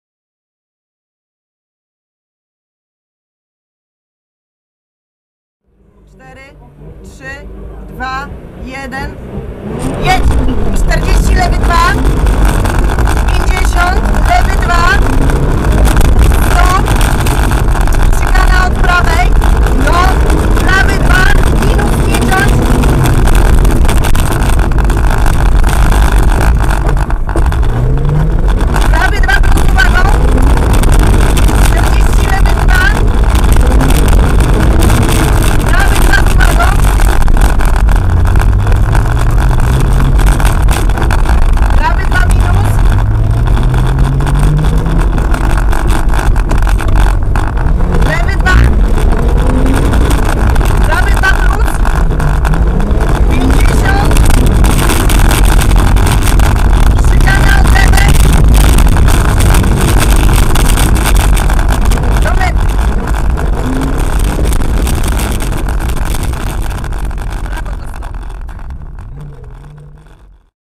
42 Rajd Monte Karlino PS1 Białogard - T. Buńka/A. Baryłowicz Audi A4 Quattro
Onboard z przejazdu prologu (PS1) 42 Rajdu Monte Karlino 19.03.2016 w Białogardzie.
Tomasz Buńka Agnieszka Baryłowicz
Audi A4 Quattro 2,8 30V